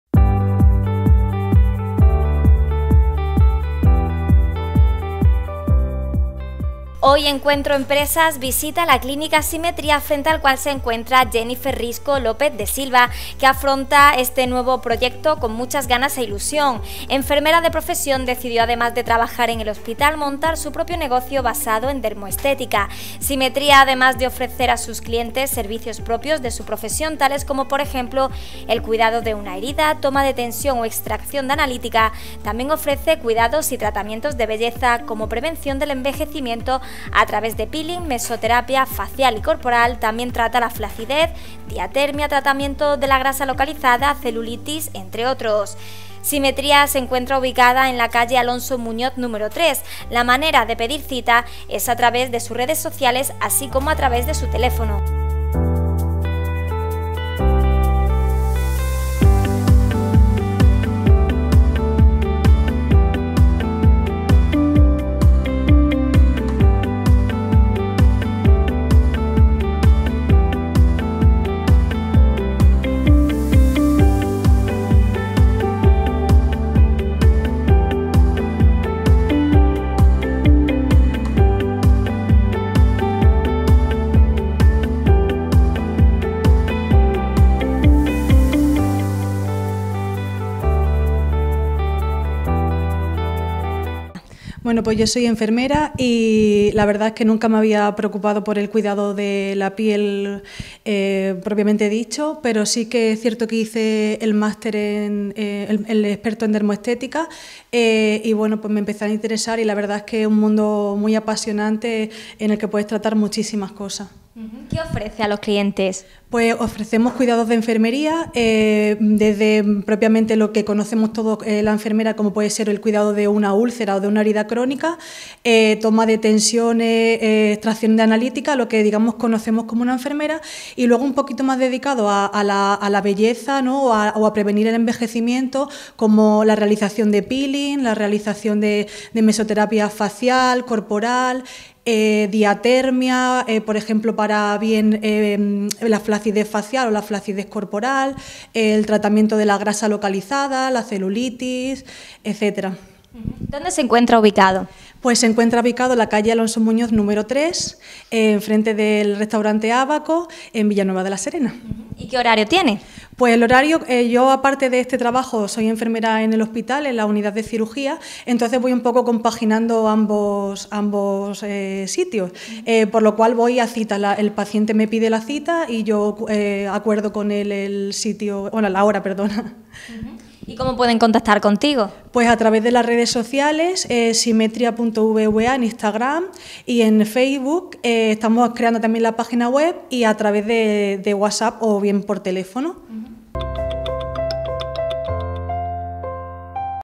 EncuentroEmpresas visita la clínica Symmetria frente al cual está Jennifer Risco López de Silva.
Hoy EncuentroEmpresas visita la clínica Symmetria frente al cual se encuentra Jennifer Risco López de Silva que afronta este nuevo proyecto con ganas e ilusión. Enfermera de profesión, decidió además de trabajar en el hospital, montar su propio negocio basado en dermoestética. Symmetria además de ofrecer a sus clientes servicios propios de su profesión como el cuidado de una herida, toma de tensión o extracción de analítica, también ofrece cuidados y tratamientos de belleza como prevención del envejecimiento a través de peeling, mesoterapia facial y corporal, flacidez, diatermia, tratamiento de la grasa localizada, celulitis…